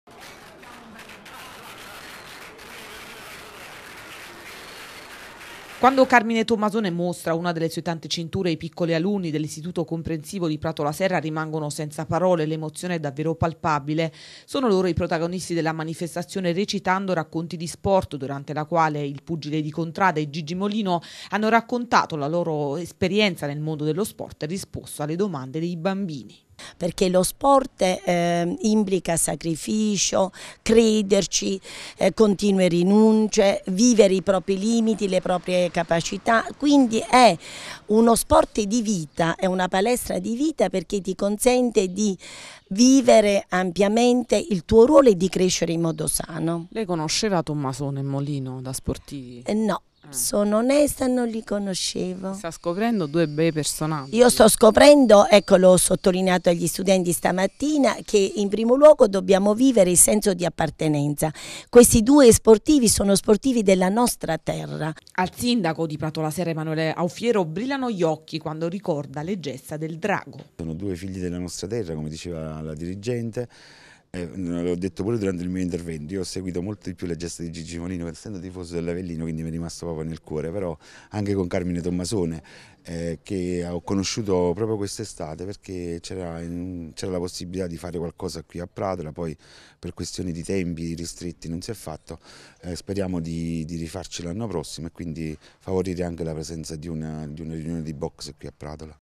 Pratola Serra, Molino e Tommasone si raccontano
L'ex attaccante dell'Avellino e il pugile di Contrada sono stati i protagonisti della iniziativa organizzata dall'Istituto Comprensivo di Pratola Serra e hanno risposto alle tante domande dei piccoli alunni.